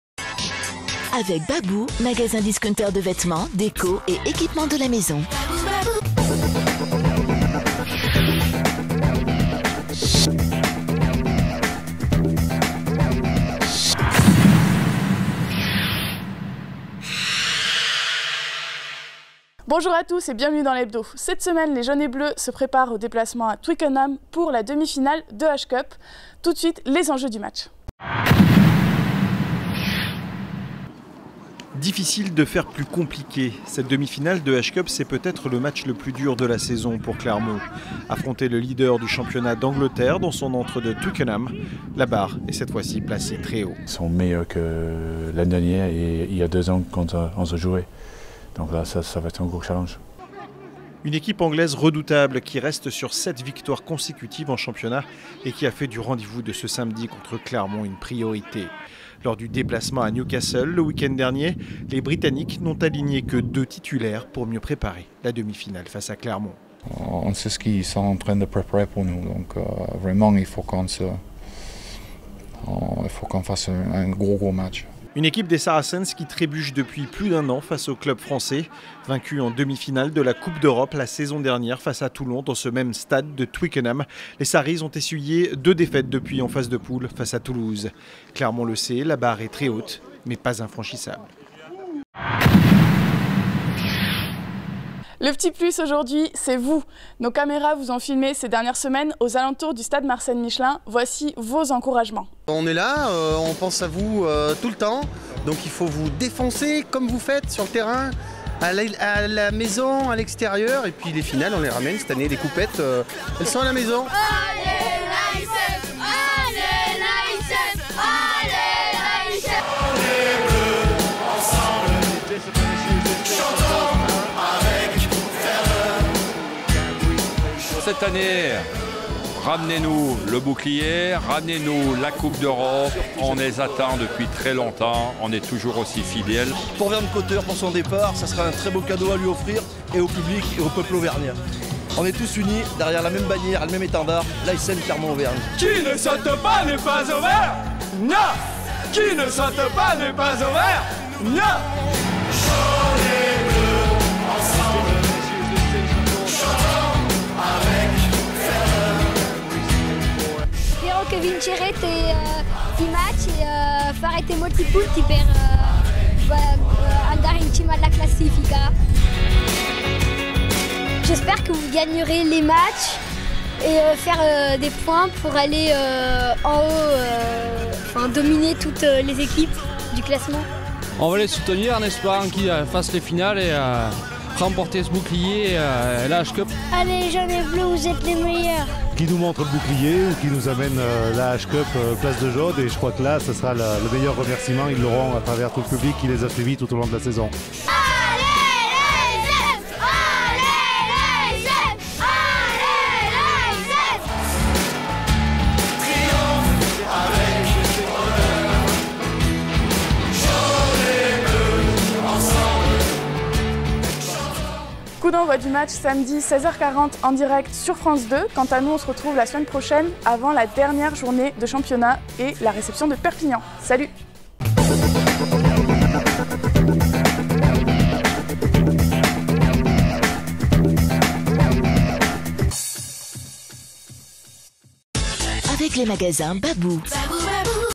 hebdo ASM : avant la 1/2 finale de H Cup
Troisième demi-finale de H Cup consécutive pour Clermont samedi à Twickenham. l'Hebdo revient sur les enjeux de cette rencontre face aux Saracens.
Dans le P'tit Plus, vos messages d'encouragement à l'équipe.